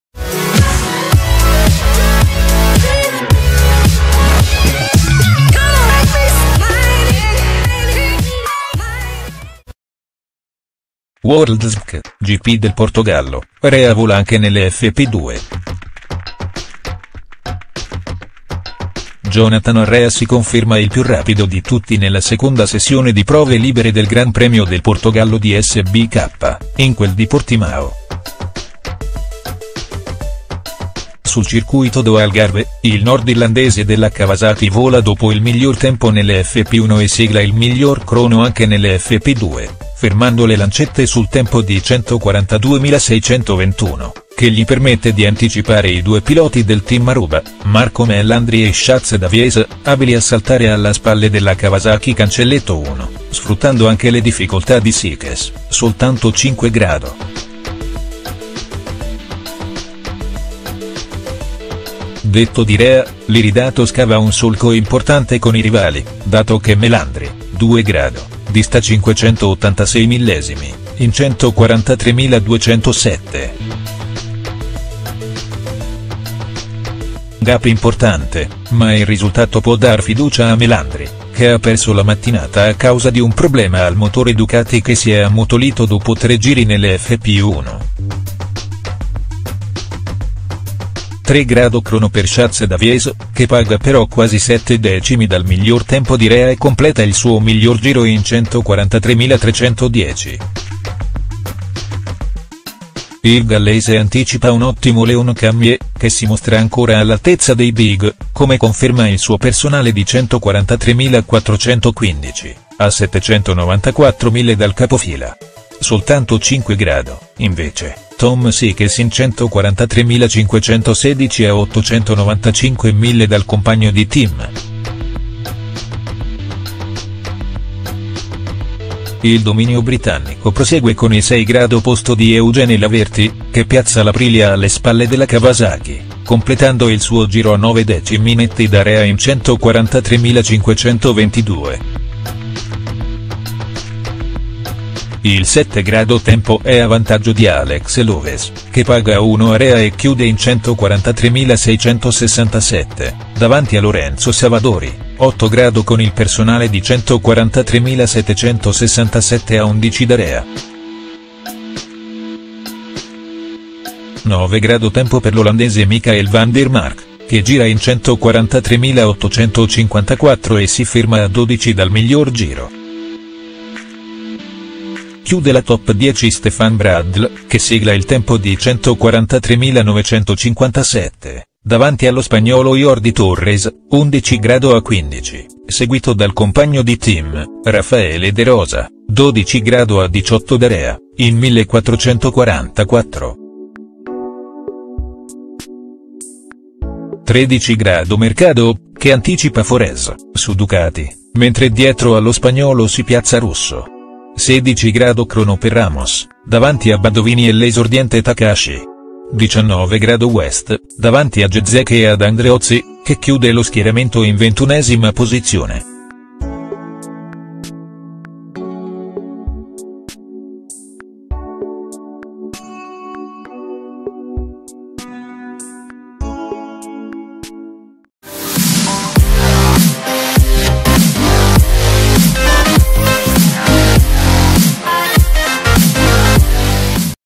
WorldSBK, Gp del Portogallo - Rea vola anche nelle FP2.HD
IL campione del Mondo si conferma il più veloce, volando in 1'42"621. Alle sue spalle chiudono le Ducati di Melandri e Davies, ad oltre 5 decimi, mentre Sykes è solo 5°.